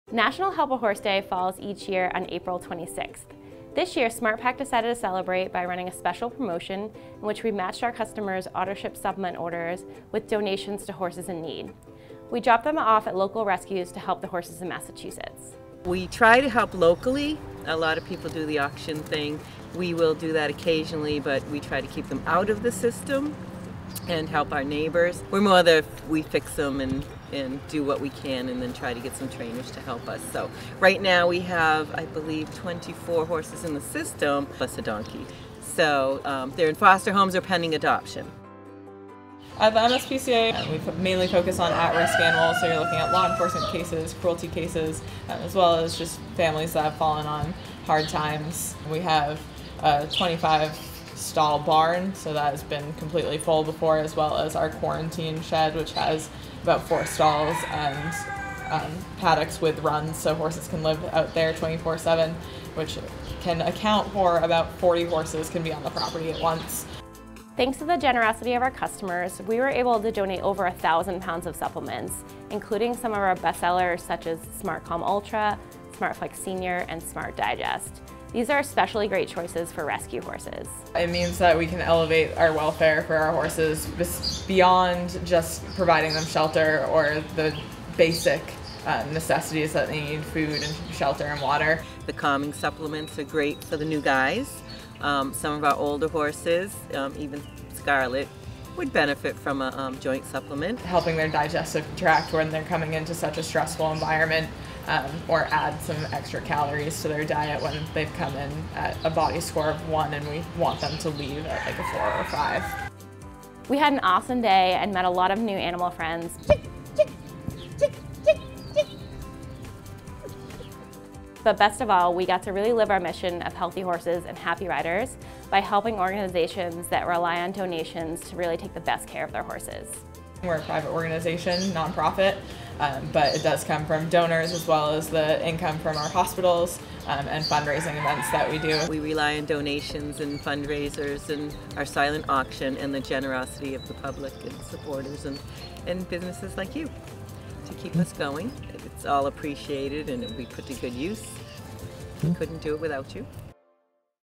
SmartPak gives back on Help a Horse Day
In honor of National Help a Horse day on April 26th, SmartPak ran a promotion that matched supplement orders from our customers with supplement donations. Thanks to all of our customers who participated, we were able to donate over 1,000 pounds of product to local non-profit organizations - all of which rely on gifts like these for their continued success.